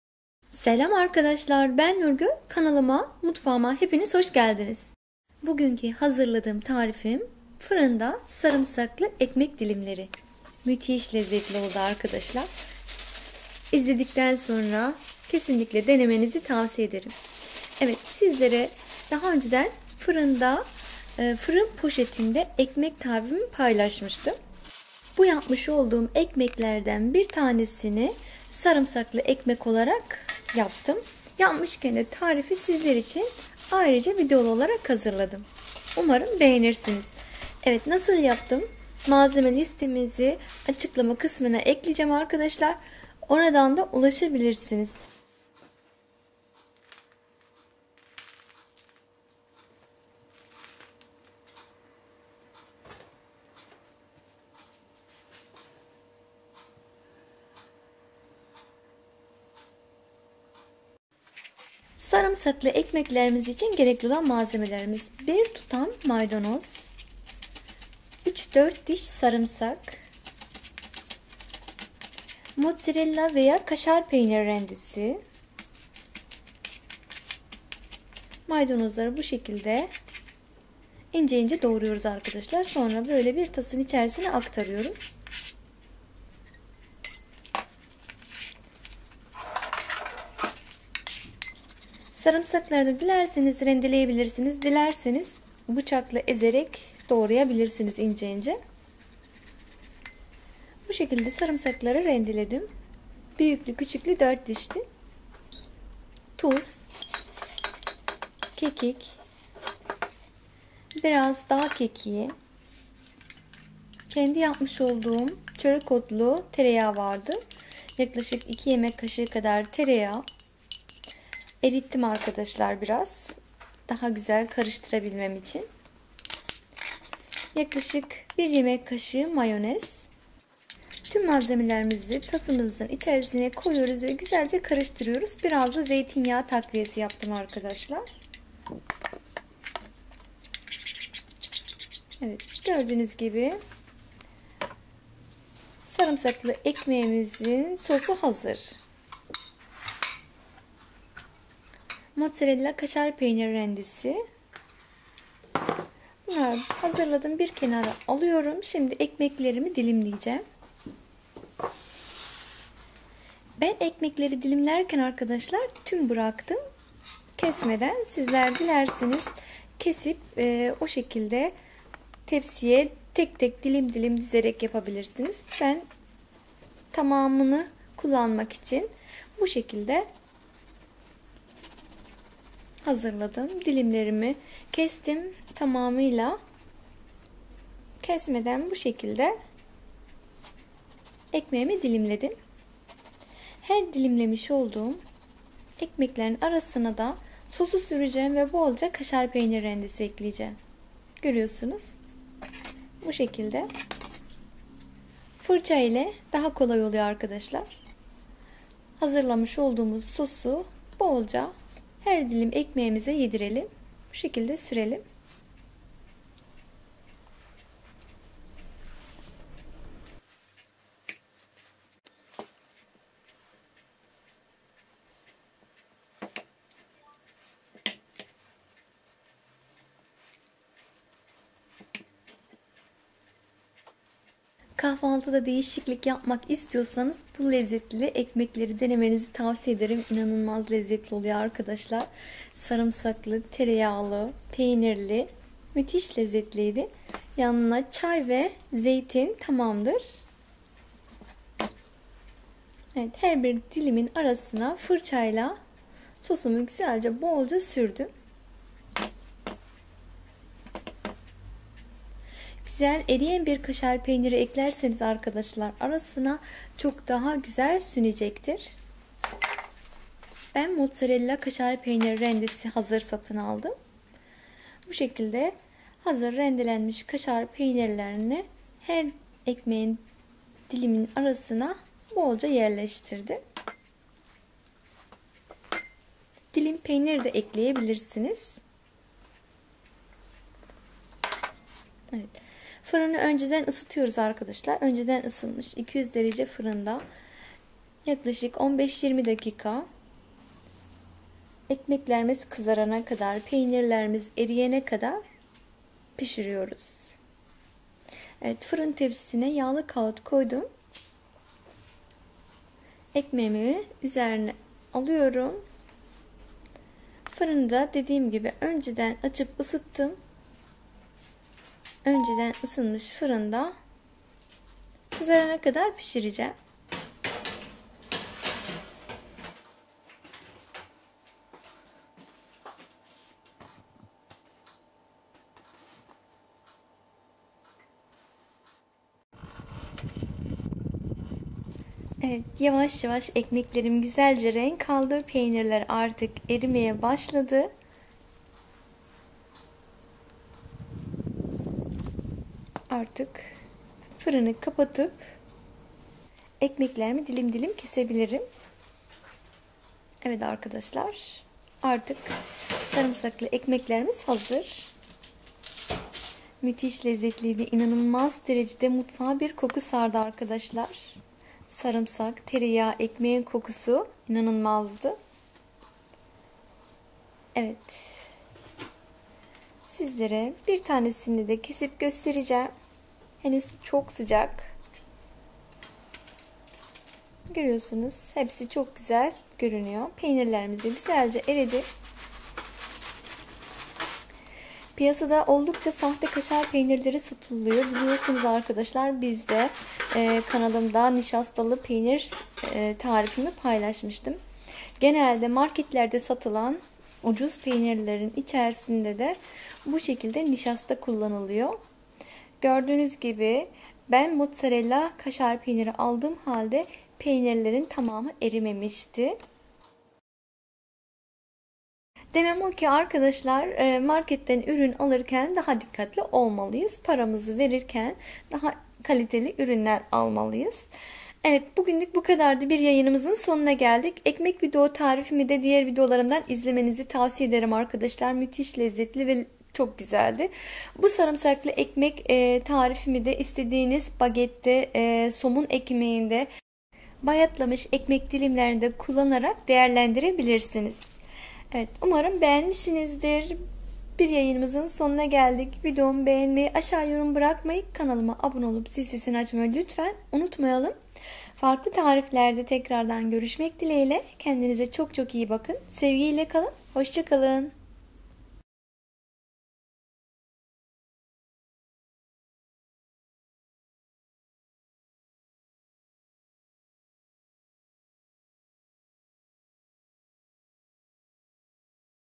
Bayat ekmekleri bu tarifle değerlendirin kapış kapış bitecek 💯✅ Sarımsaklı Ekmek Nurmutfagi NurGüL
SARIMSAKLI EKMEK

Malzemeler
2 yemek kasik eritilmis tereyag
bir tutam maydanoz
3-4 dis sarimsak
1 yemek kasik mayonez
2-3 yemek kasik zeytinyag
tuz, kekik, dagkekigi